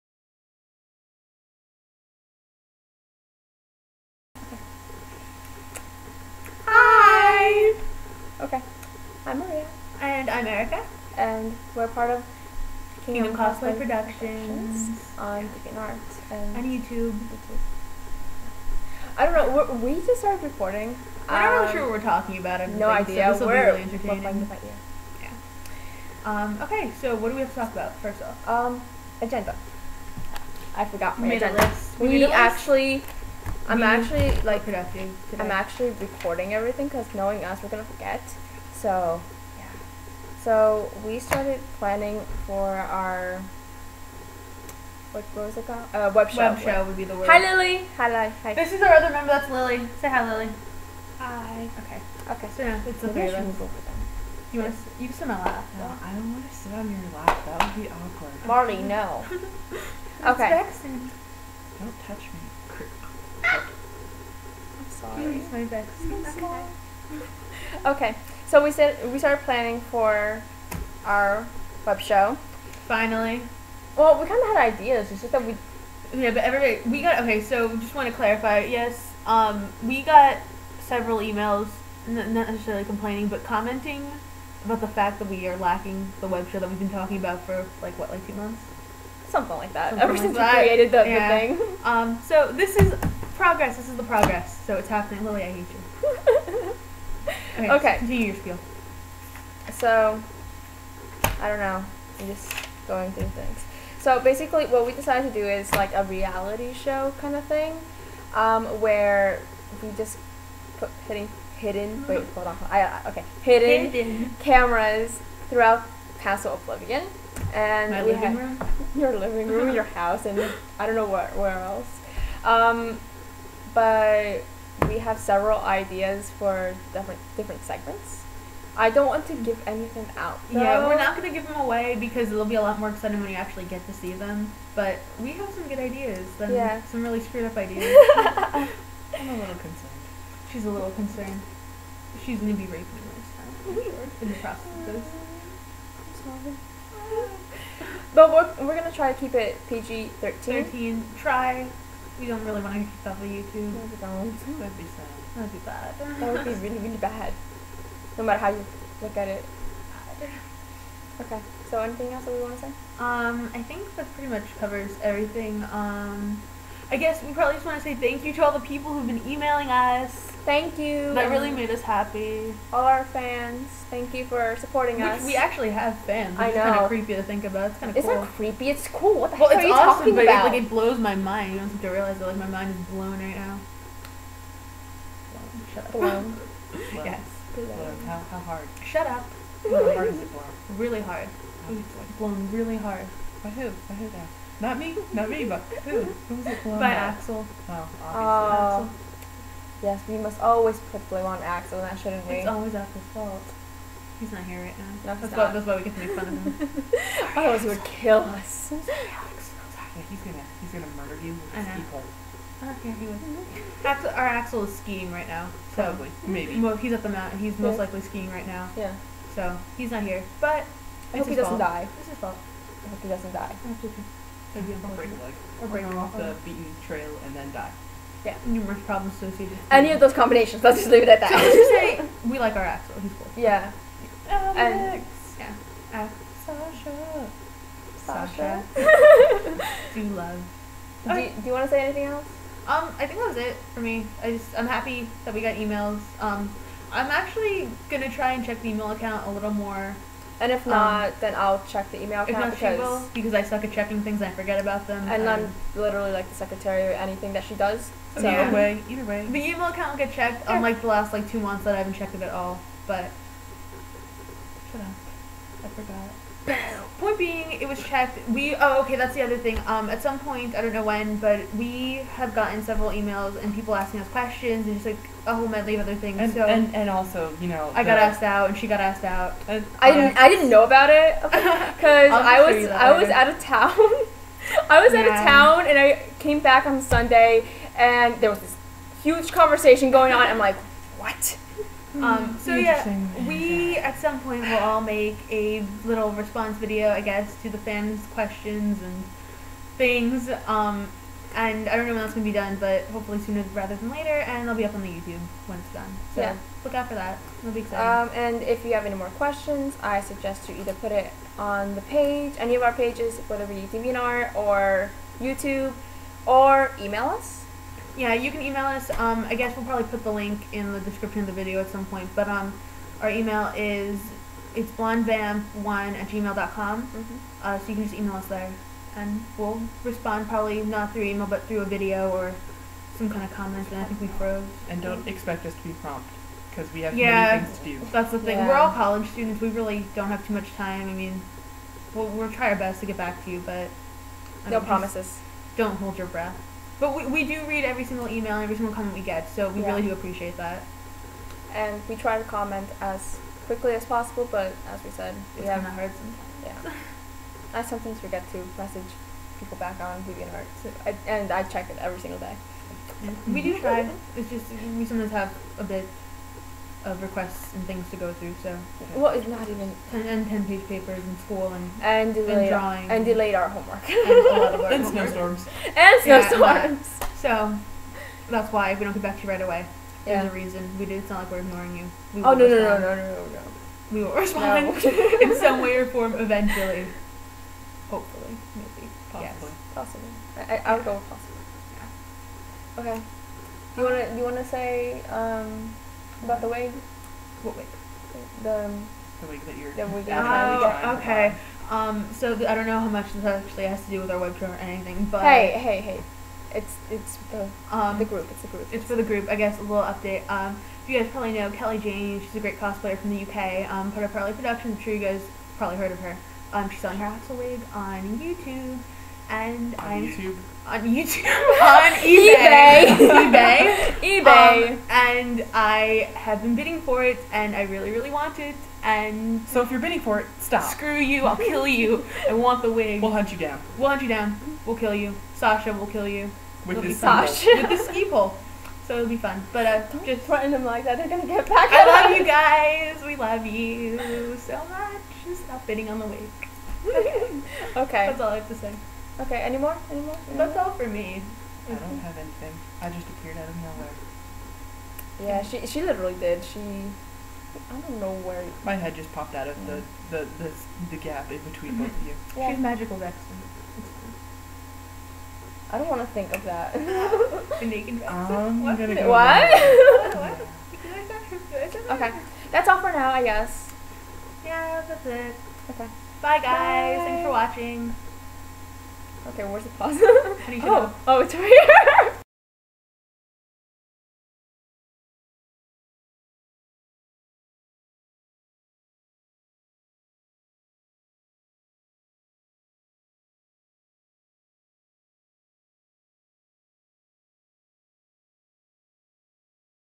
A Retarded Update
Lily, Erica and Maria talk about very interesting subjects including new Kingdom Cosplay Production plans and other miscellaneous topics.